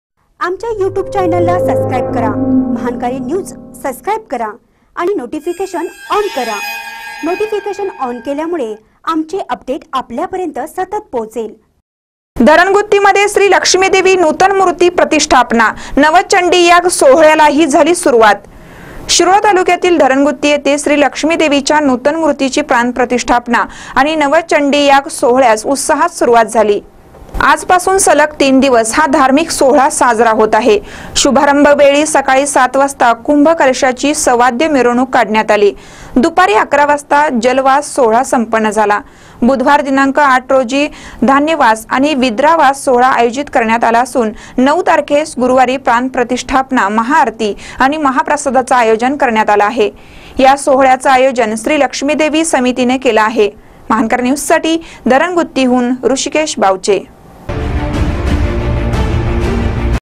धरणगुत्तीमध्ये श्री लक्ष्मीदेवी नूतन मूर्ती प्राणप्रतिष्ठा
धरणगुत्तीमध्ये श्री लक्ष्मीदेवी नूतन मूर्ती प्राणप्रतिष्ठा : नवचंडियाग सोहळ्यालाही झाली सुरवात